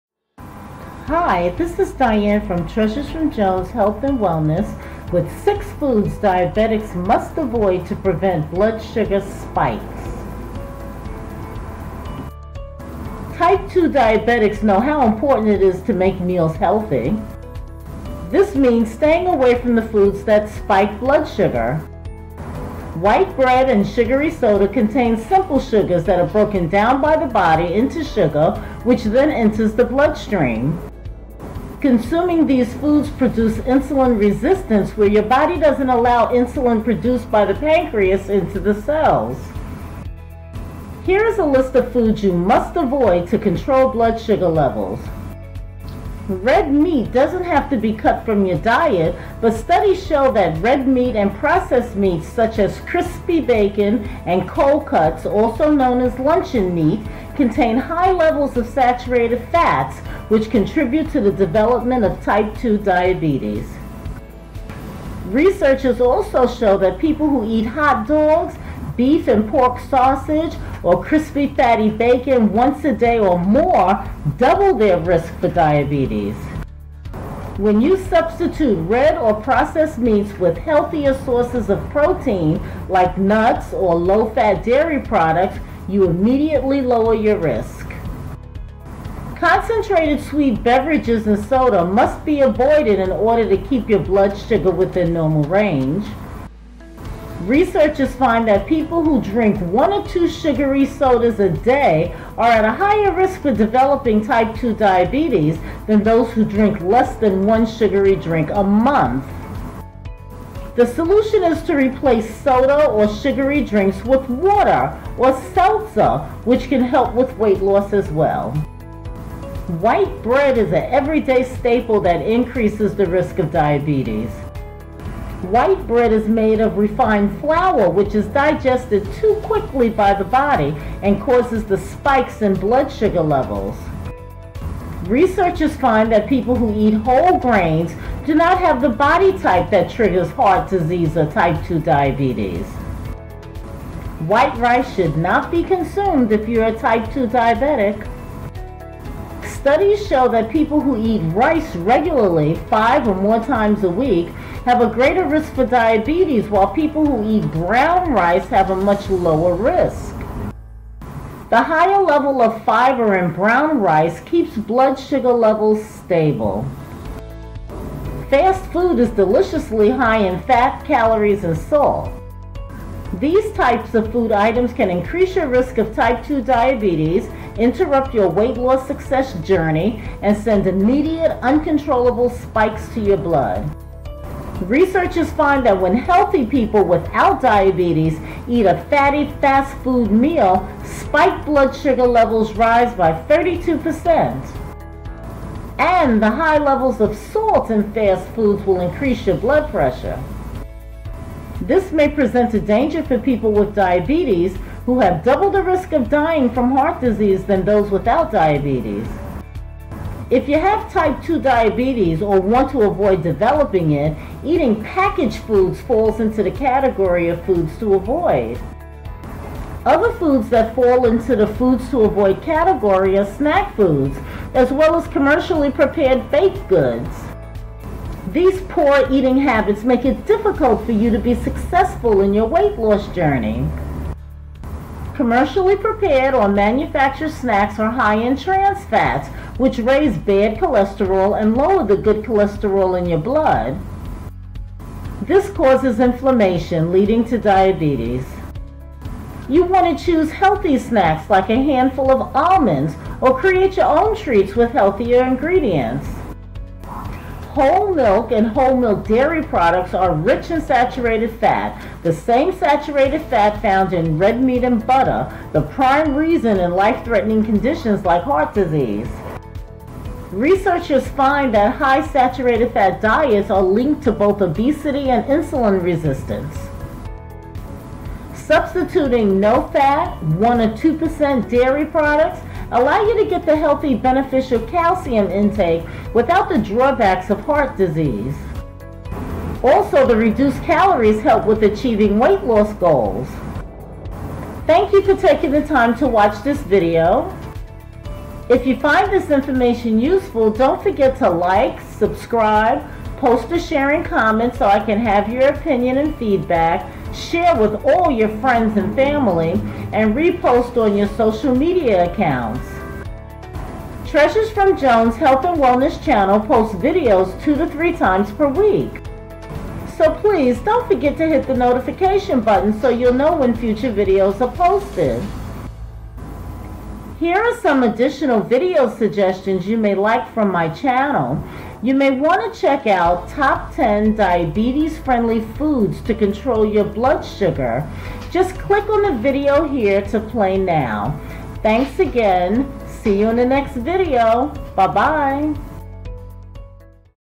6 Worst Foods for Diabetes | Control Blood Sugar
6 Worst Foods for Diabetes | Control Blood Sugar.  Type 2 diabetics must limit or avoid the worst foods that cause high sugar levels and spike blood sugar and promote health risks. These 6 foods will help you control blood sugar levels.

1.  Red Meat doesn't have to be cut from your diet, but studies show that red meat, and processed meats, such as crispy bacon, and cold cuts aka luncheon meat, contain high levels of saturated fats, which contribute to the development of type 2 diabetes.

2.  Concentrated sweet beverages and soda, must be avoided in order to keep your blood glucose sugar, within normal range.

3.  White Bread is an everyday staple, that increases the risk of diabetes.

4.  White Rice should not be consumed if you are type 2 diabetic. Brown rice, keeps blood sugar levels stable.

5.  Fast food is deliciously high in fat, calories and salt and increase your risk of type 2 diabetes, if you have type 2 diabetes or want to avoid developing it, eating packaged foods falls into the category of foods to avoid.

6.  Fresh whole milk, and whole milk dairy products are rich in saturated fat which is linked to both obesity, and insulin resistance.